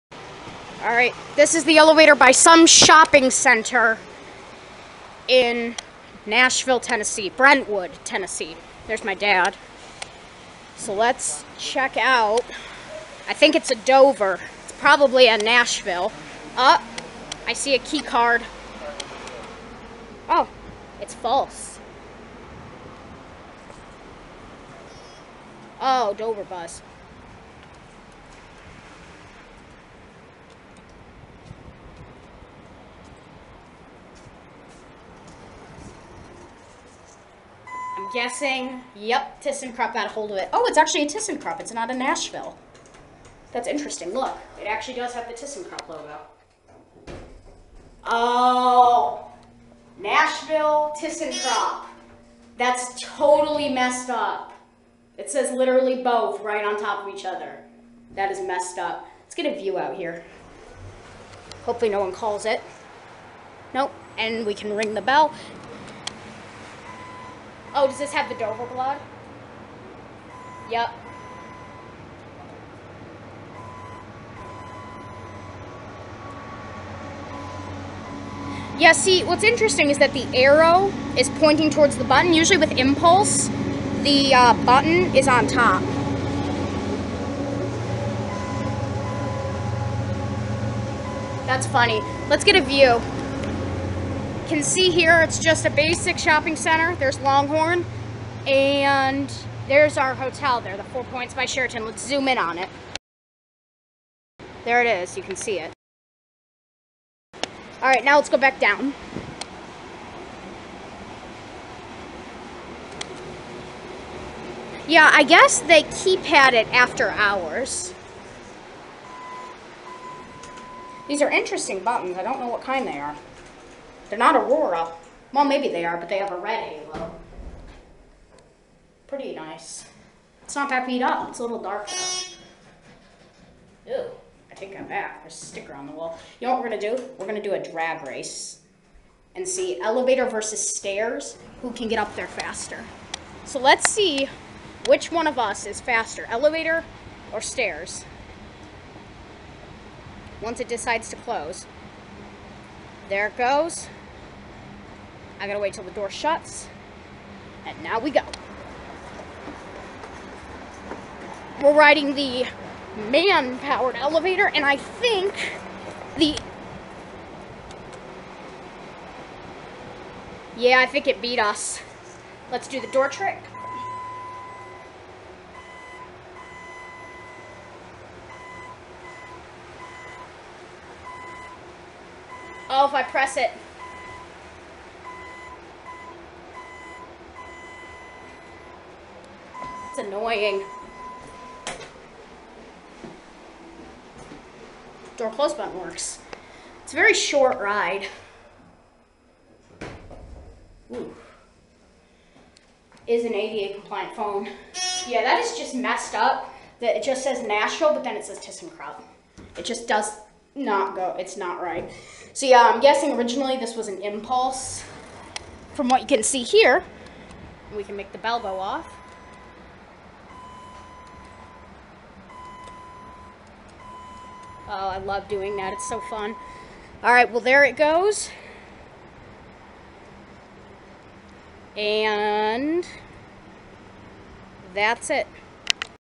ThyssenKrupp/Nashville Hydraulic Elevator at The Shoppes At Brentwood in Brentwood TN
Interesting elevator. It obviously had impulse originally. I thought it was funny that ThyssenKrupp's name was right above the "NASHVILLE MACHINE ELEVATOR CO." logo. This was taken on my camera.